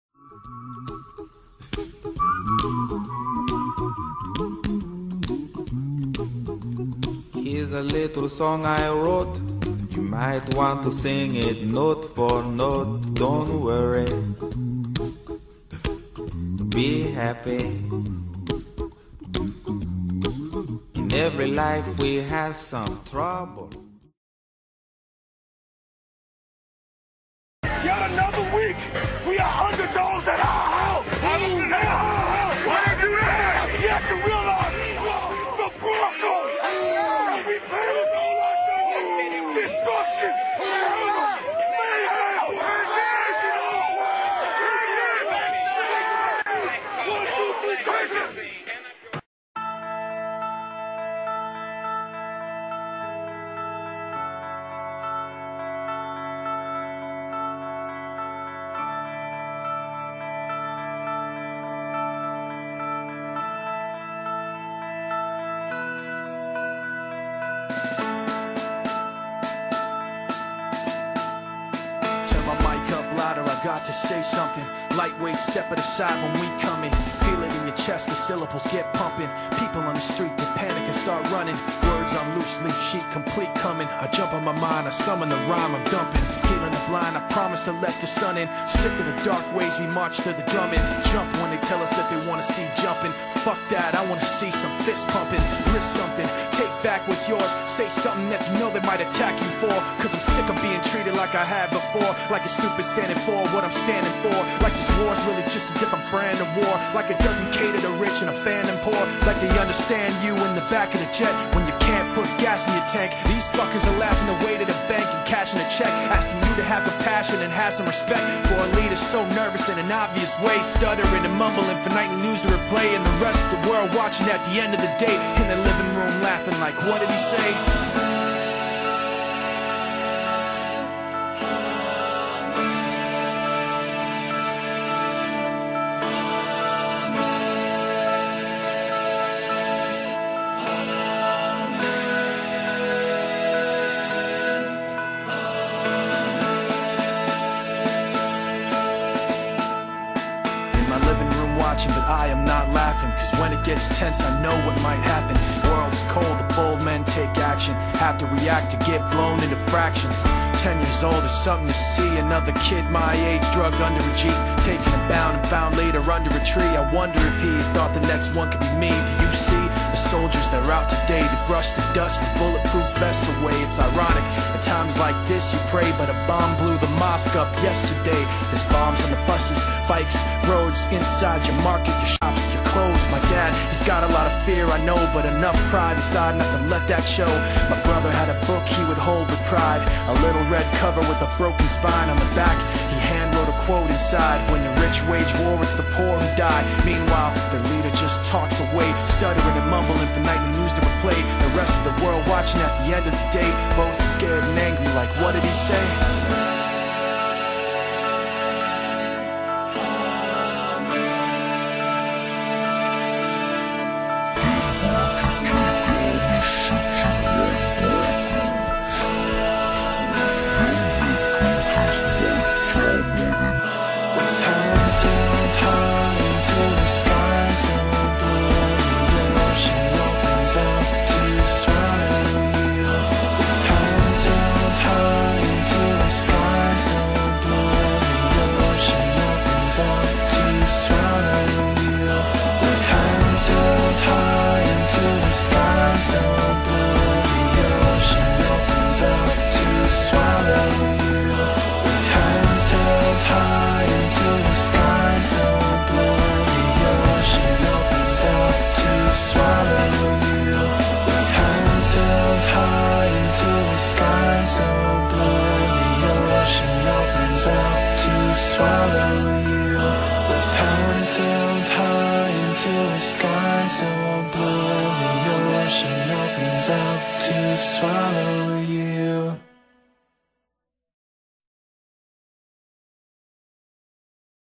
2009 Denver Broncos
Highlights of the 2009 Denver Broncos and their Road to success.